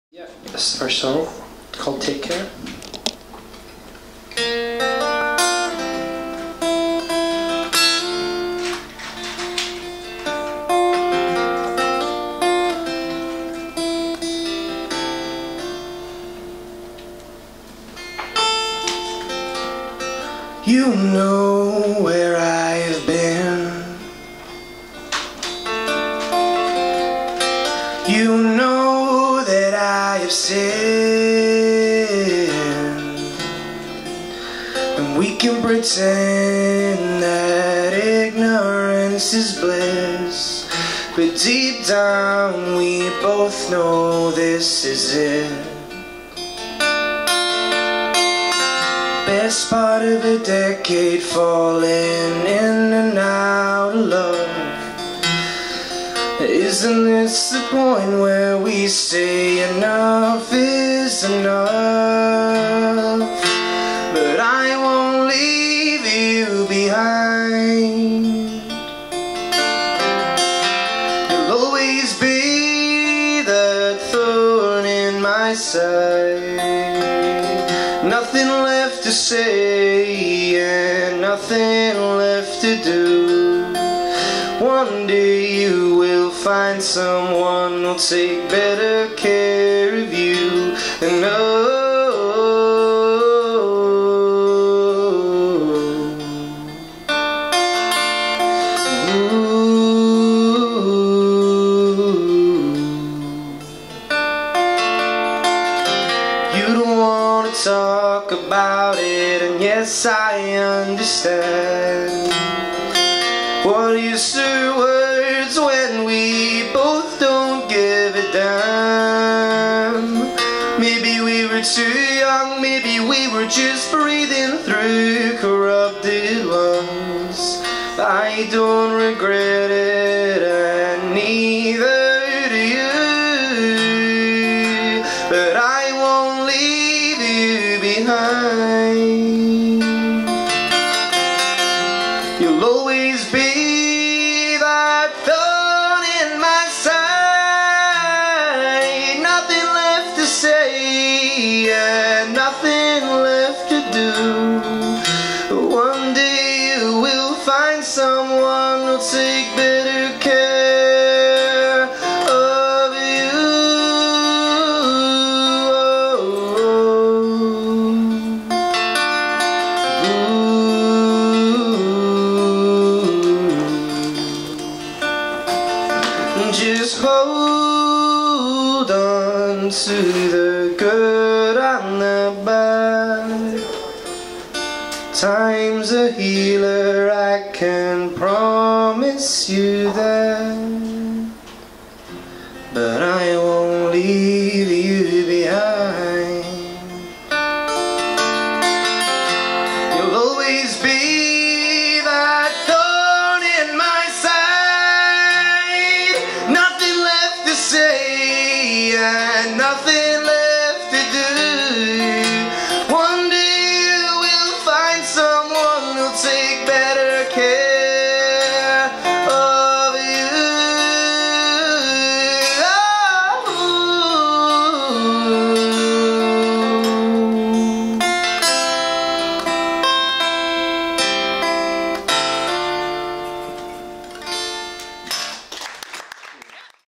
Rossa Murray @ Naked Guitar Acoustic Night in Attic
Every other Wednesday in Attic, Grethe Borsum presents some of Liverpool's brilliant and eclectic acoustic artists. Come down for a beer and see what it's all about!